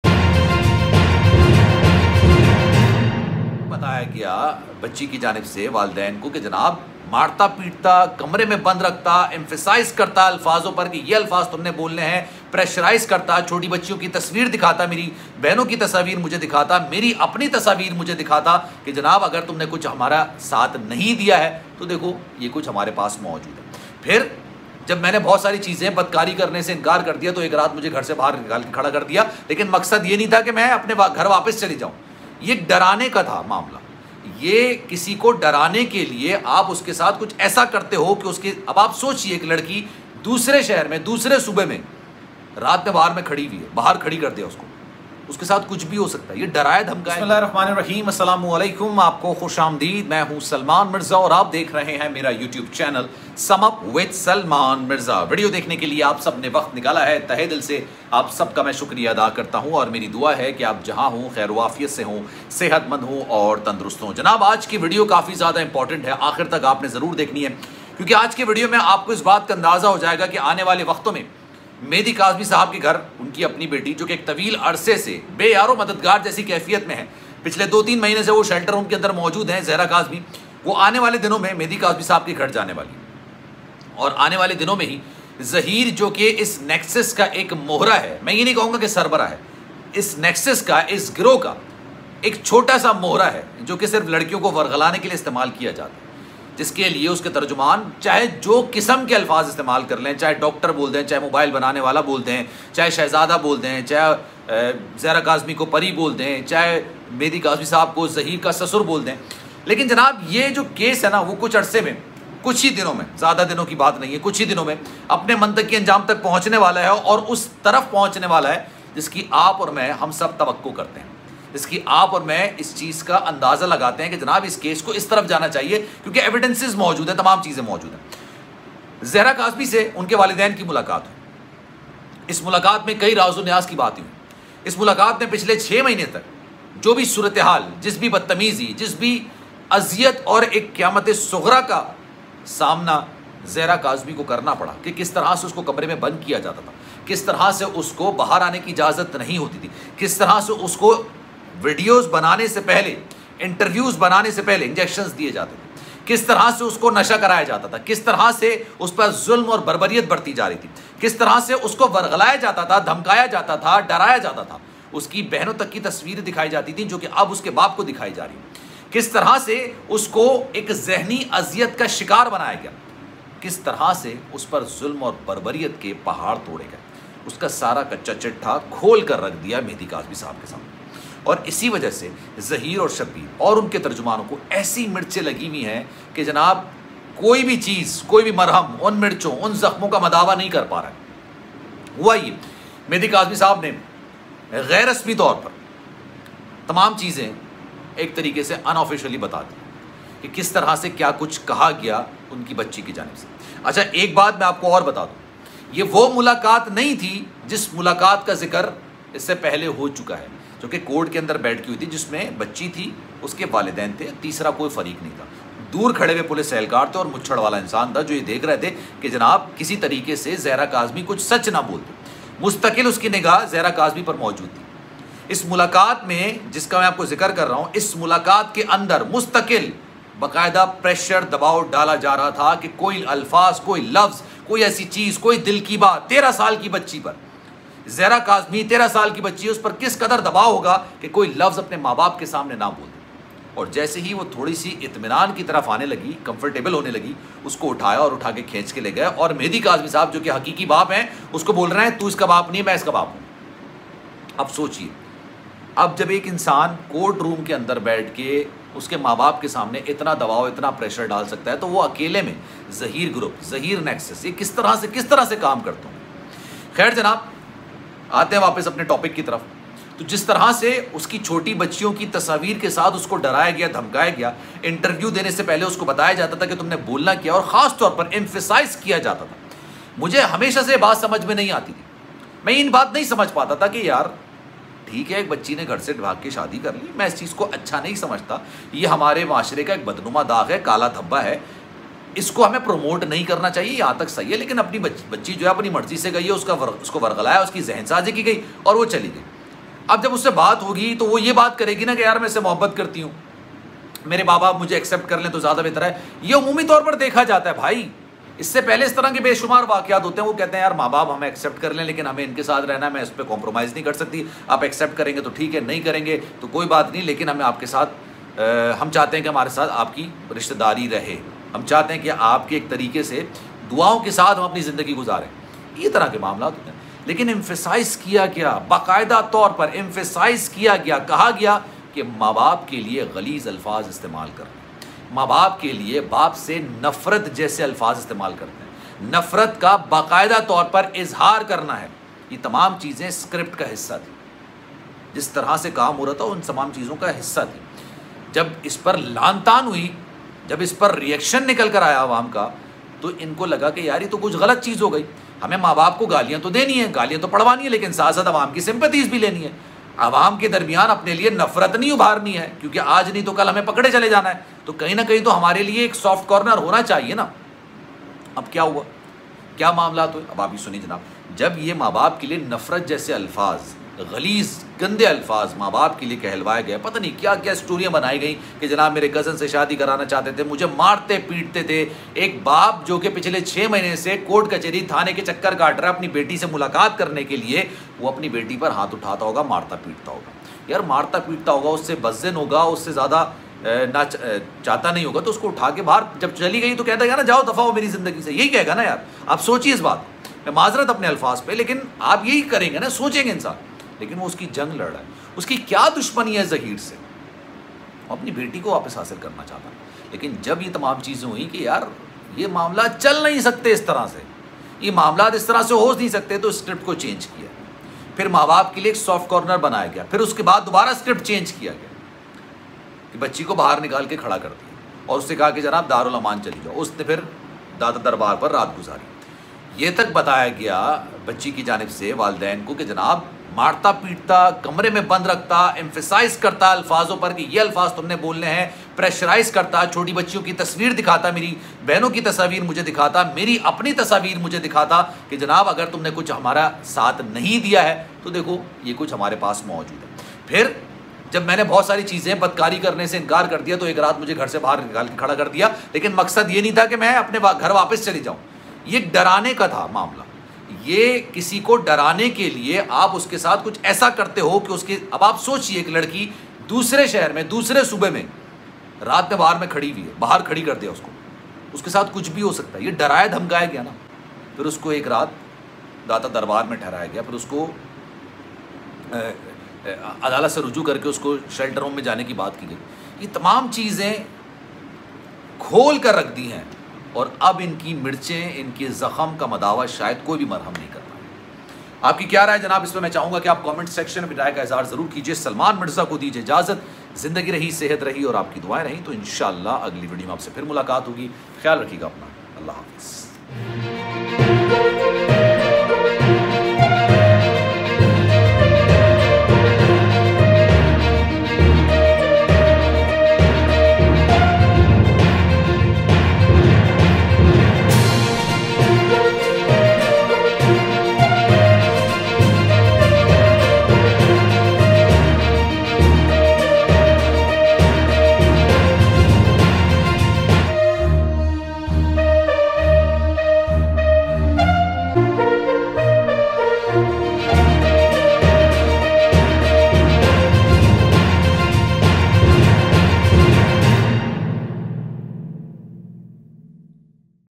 Dua zahra ki videos karney ki dhamki || Voice note samney agaya || Salman Mirza official
Dated:  20-October-2022